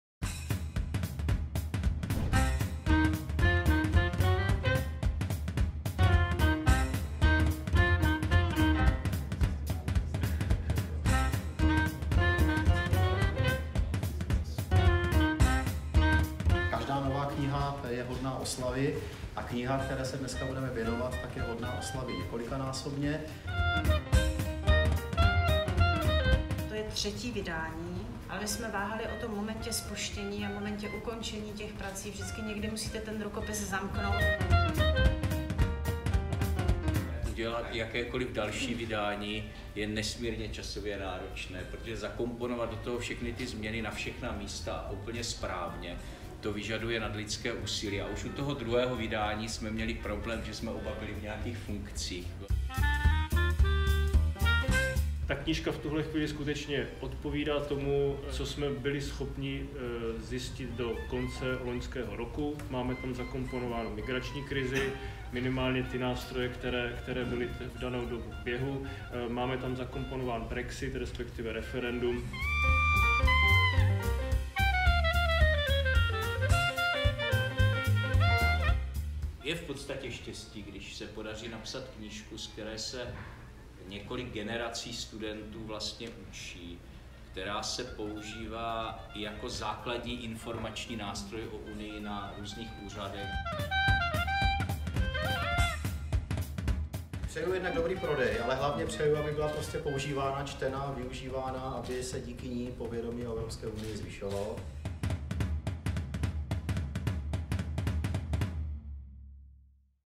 European Union: The Third Edition - Book Presentation (2018)
On December 3, I participated in a book presentation and signing event at Faculty of Social Studies of Masaryk University. My dad, Markéta Pitrová and Ondřej Krutílek presented the third updated edition of the most comprehensive book ever written on the subject of European Union. This short video serves both as a recap of the event and as an trailer for the book itself.

Video was shot on iPhone X with no extra lens attached, using the FiLMiC Pro app + DJI Osmo Mobile 2.